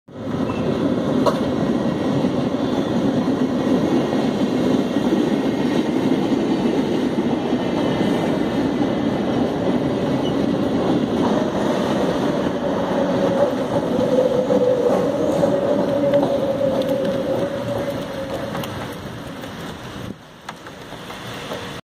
1700 CPTM 1743
Chegando na estação Água Branca Sentido FCO Morato, linha 7 Rubi.